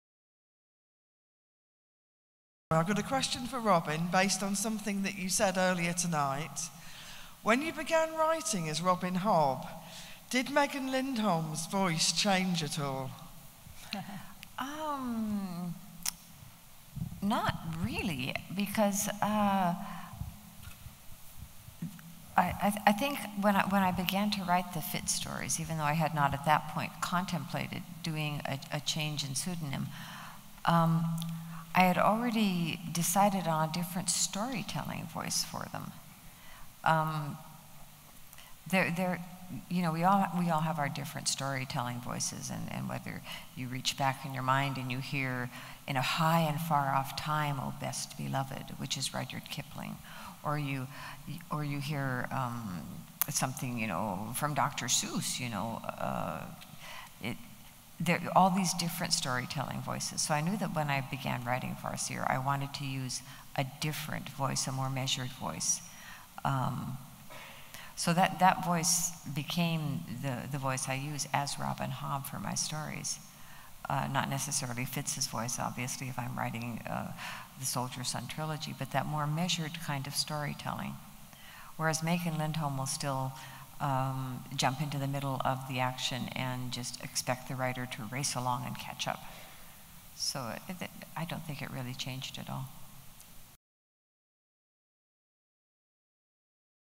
Robin Hobb on her pseudonyms & storytelling voices.
Robin Hobb answers a fan question regarding her variety of pseudonyms & how these changes can affect her 'voice' in storytelling.

Robin was speaking alongside George R. R. Martin at a world exclusive event which blinkbox Books were proud to sponsor.